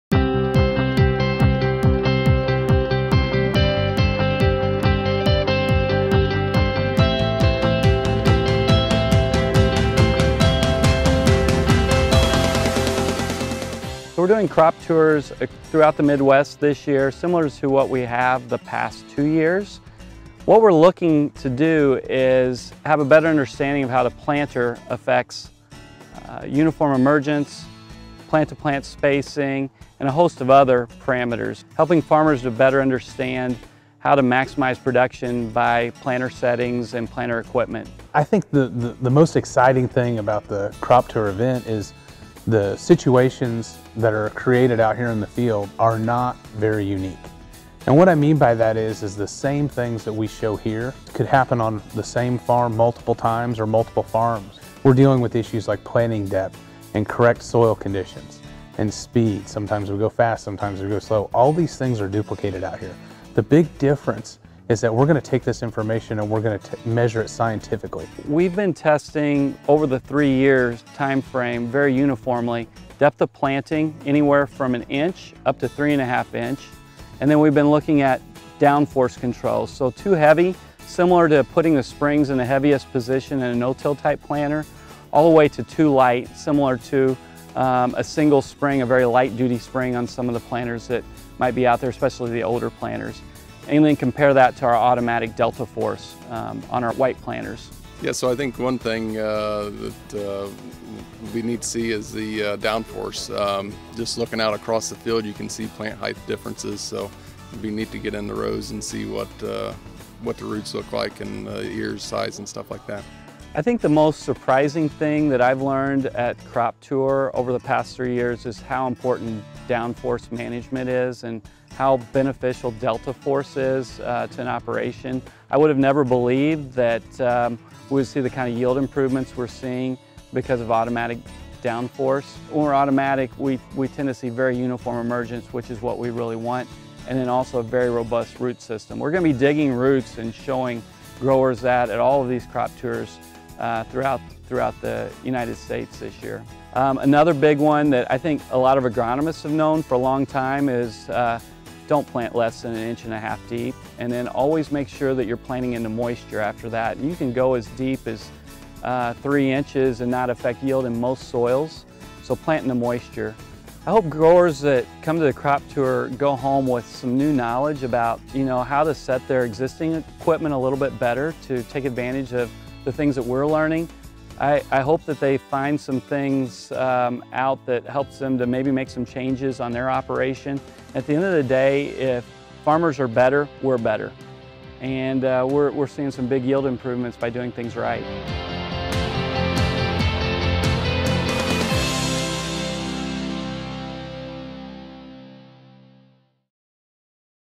Planting Depth and Down Force - Insights from AGCO Crop Tour North America 2018
The 2018 AGCO Crop Tour in North America invited growers to 10 U.S. locations to see for themselves how best planting practices--like proper planting depth and the right amount of down force--can boost corn yields. Darren Goebel, AGCO director, global agronomy and farm services, and Matt Bennett, sales lead for Precision Planting, discuss what was learned in the third year of the Crop Tour.